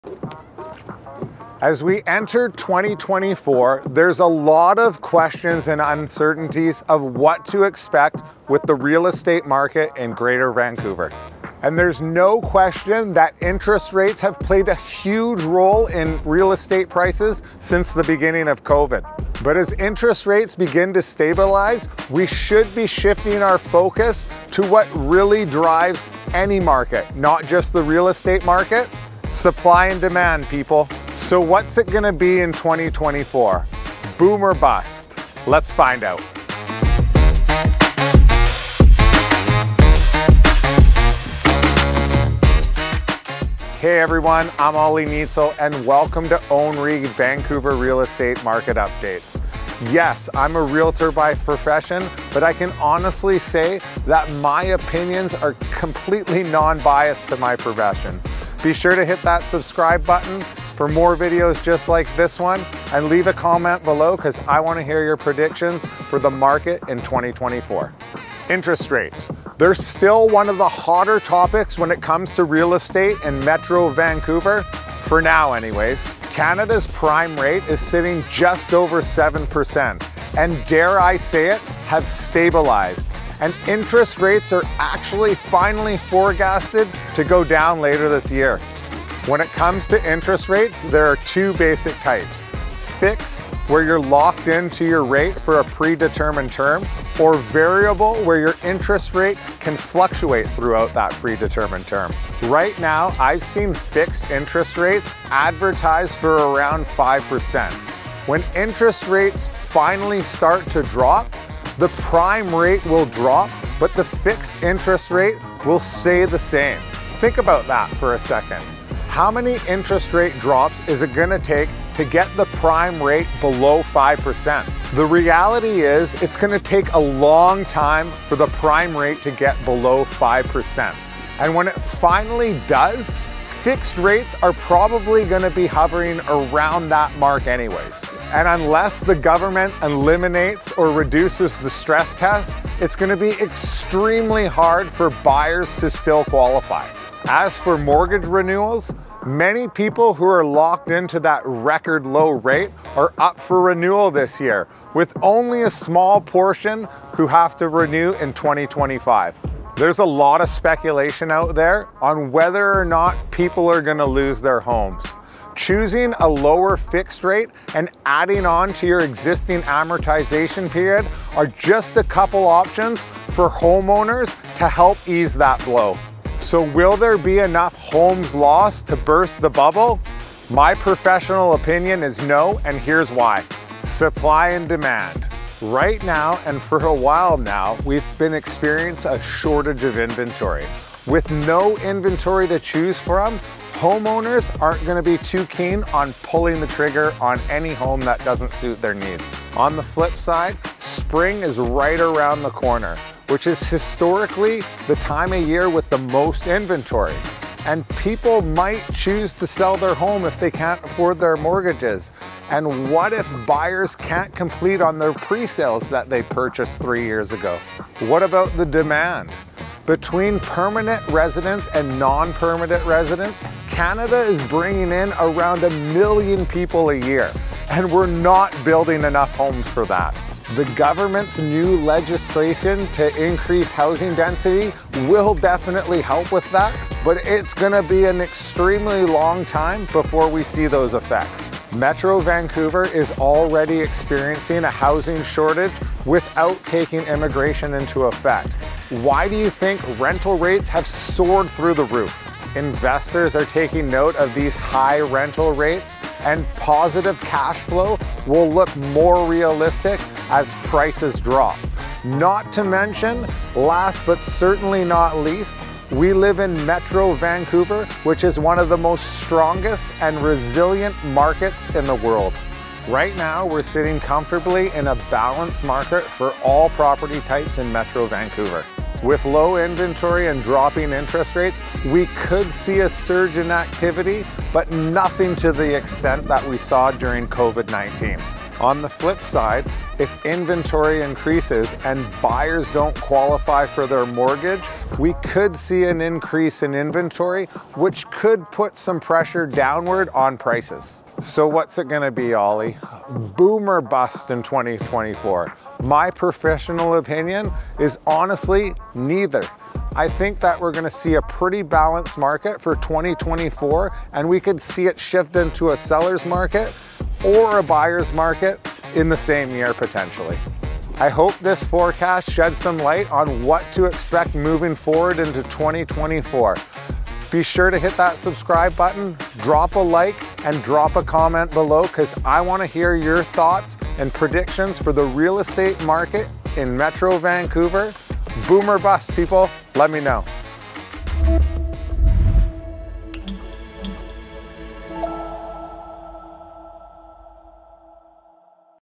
Vancouver Real Estate Market Forecast 2024
As we enter 2024 there are a lot of questions and uncertainty of what to expect with the Real Estate Market in Metro Vancouver. It’s no question that interest rates have a played a key role with the rise and fall of real estate market prices since the beginning of COVID.  But as interest rates begin to stabilize, our focus should be shifting back to what drives not just the Real Estate Market but any market. Supply and demand!

So, what’s in going to be in 2024? Boom or bust?

2021 Recognition Awards:

Top 2 Individual -  Royal Lepage West R.E.S
Top 2% in Canada - Royal Lepage Re Diamond Award
Top 3% Greater Vancouver - Medallion Club Member